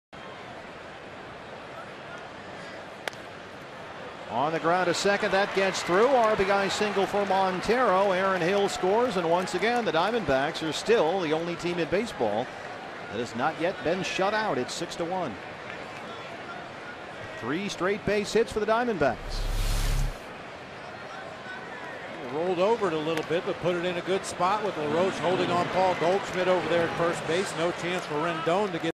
Montero singles home the D-backs' first run
6/25/13: Miguel Montero knocks a base hit through the right side of the infield, scoring Aaron Hill with the D-backs' first run of the game